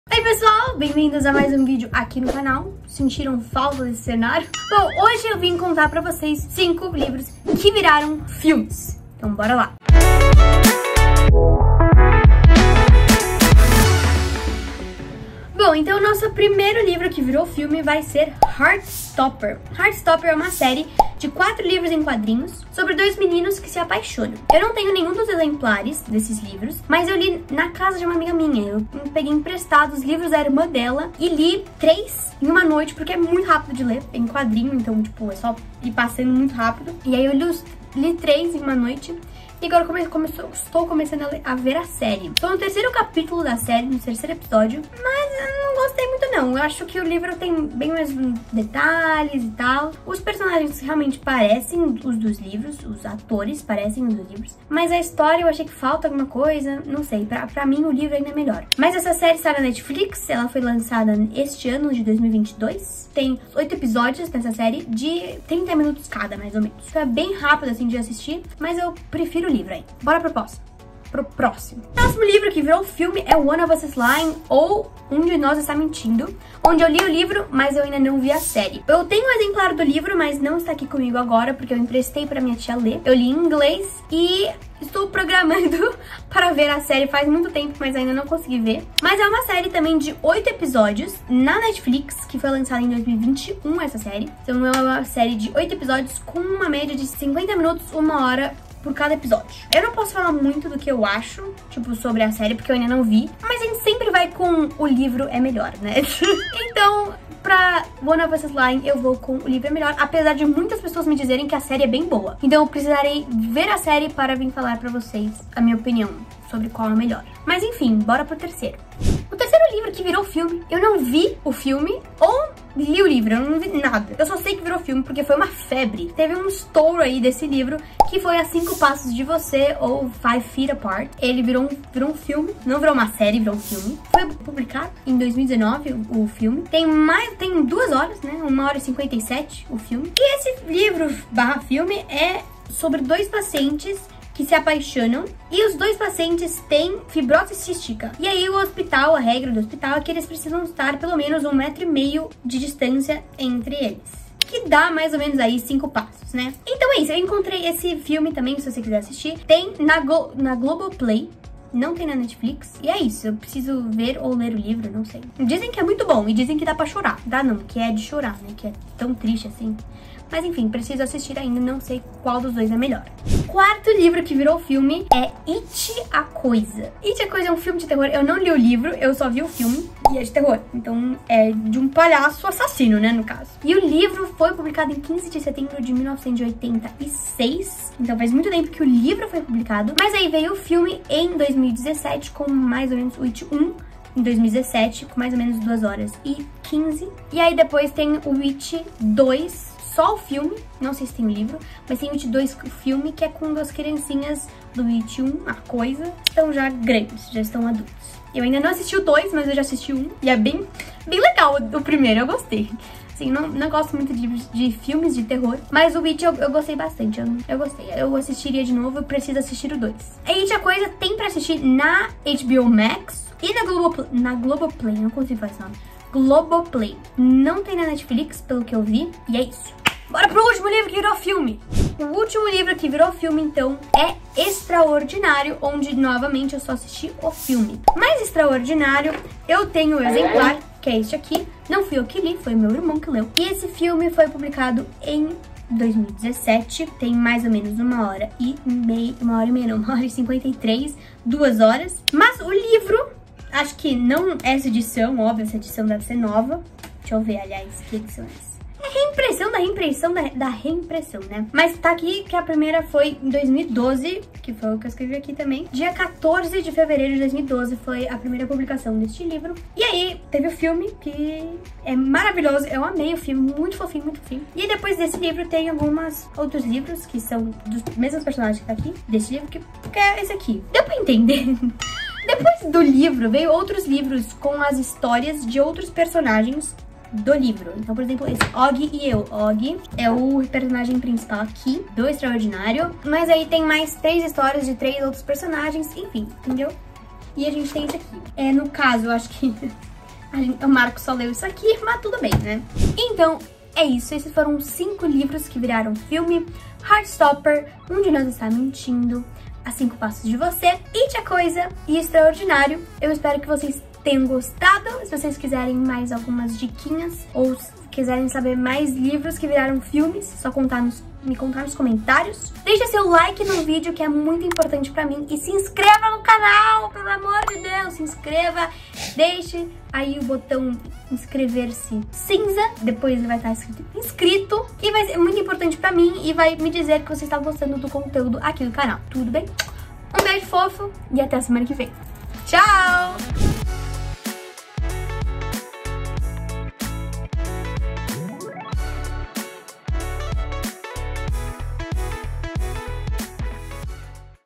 5 LIVROS QUE VIRARAM FILMES E SÉRIES
5 LIVROS QUE VIRARAM FILMES E SÉRIES

No vídeo de hoje, eu quero mostrar a vocês 5 livros que viraram filmes ou séries.
Você gosta de séries? Gosta de filmes? Com certeza você deve saber que muitos filmes que fazem sucesso são baseados em livros.
Deve ser incrível saber que você escreveu um livro e o livro foi transformado em um filme ou uma série!
Me conta, quais filmes baseados em livros  você gosta mais? E quais séries baseadas em livros que você acompanha?

Toda semana eu trago novidades e coisas legais para os meus seguidores!

Venha comigo e chame os amigos e familiares!

Beijos Fofos.